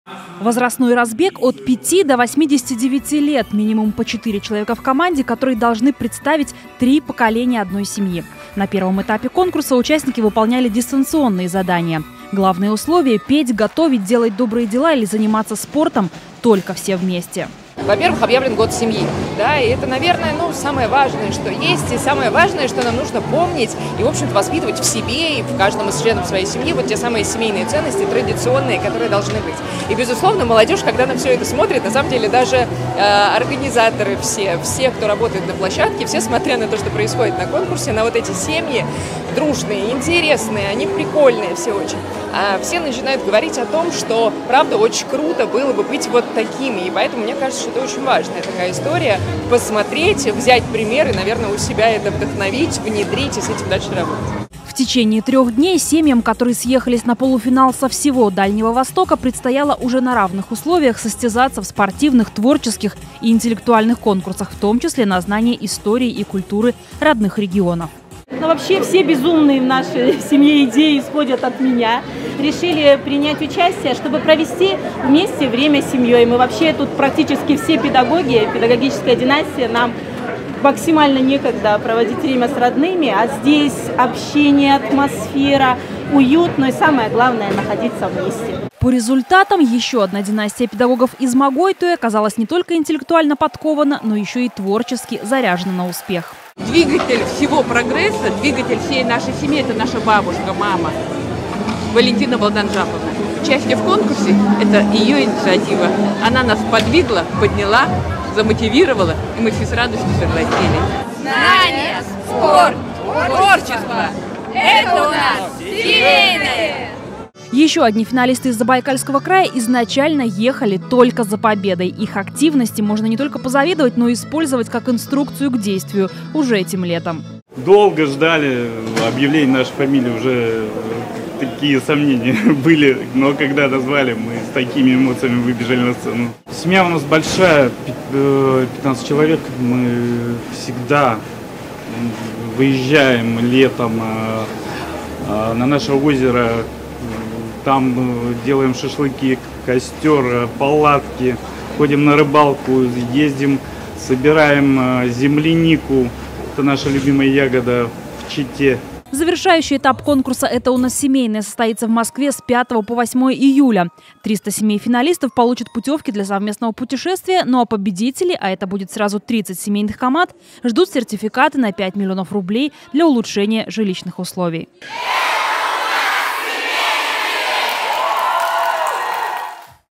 Две семьи из Забайкалья могут выиграть путешествие или 5 миллионов рублей
Две семьи из Забайкальского края вышли в финал Всероссийского конкурса «Это у нас семейное». В дальневосточном Федеральном округе  заявки на участие подали больше 28 тысяч человек и только  70 команд сумели дойти до окружного этапа. Трехдневные испытания на сплоченность завершились накануне во Владивостоке.